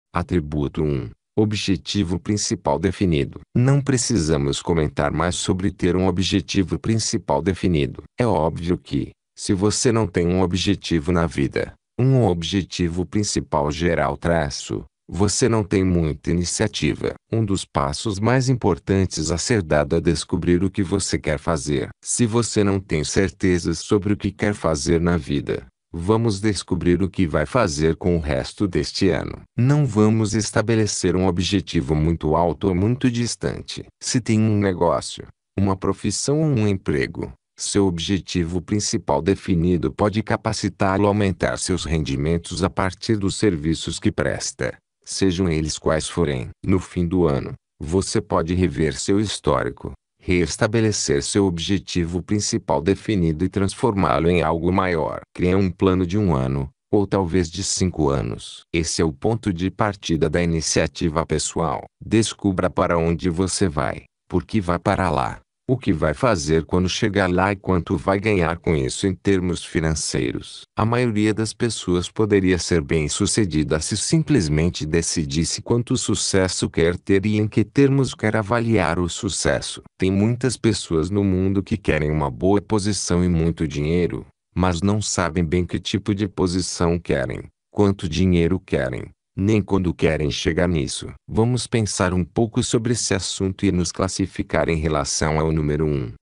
ATRIBUTO 1: OBJETIVO PRINCIPAL DEFINIDO
Não precisamos comentar mais sobre ter um objetivo principal definido.
É óbvio que, se você não tem um objetivo na vida – um objetivo principal geral –, você não tem muita iniciativa.
Um dos passos mais importantes a ser dado é descobrir o que você quer fazer.
Se você não tem certeza sobre o que quer fazer na vida, vamos descobrir o que vai fazer com o resto deste ano.
Não vamos estabelecer um objetivo muito alto ou muito distante.